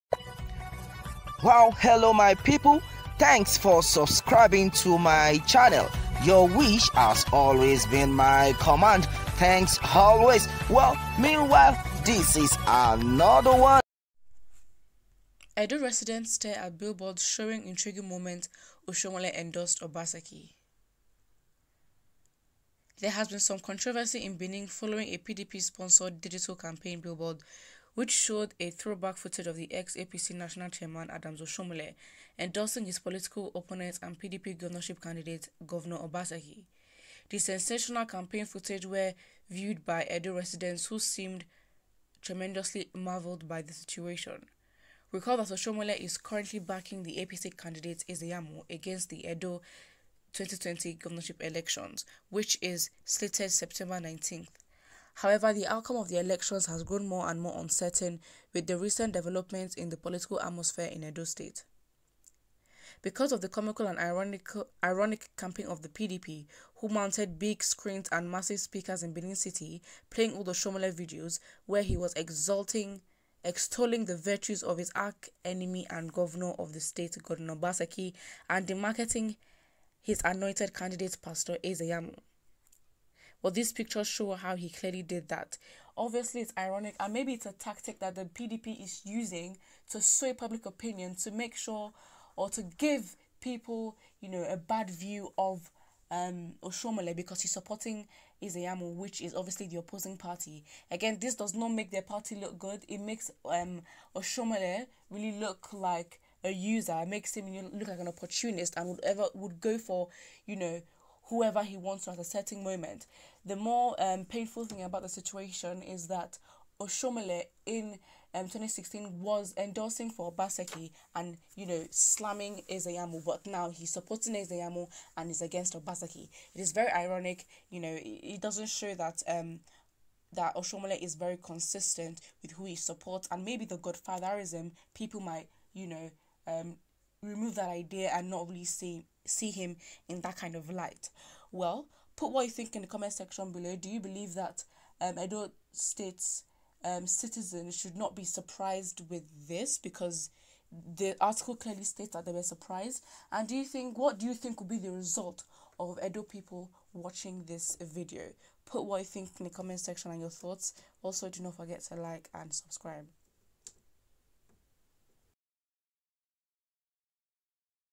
EDOLITES WATCH BILLBOARD SHOWING INTRIGUING MOMENT OSHIOMOLE ENDORSED OBASEKI
EDOLITES WATCH BILLBOARD AT RING ROAD SHOWING OSHIOMOLE ENDORSING OBASEKI